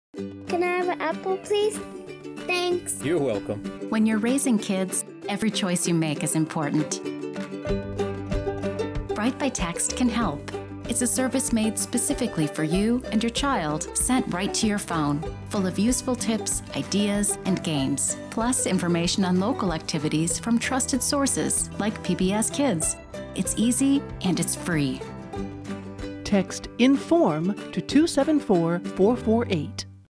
Sign up for Bright by Text from Alaska Public Media and PBS Kids
Alaska Public Media Educational Outreach has launched a new free service that sends tips and resources promoting child development via text message. Bright by Text provides quality information and trusted resources to parents and caregivers of children prenatal to five. Text INFORM to 274448 for resources!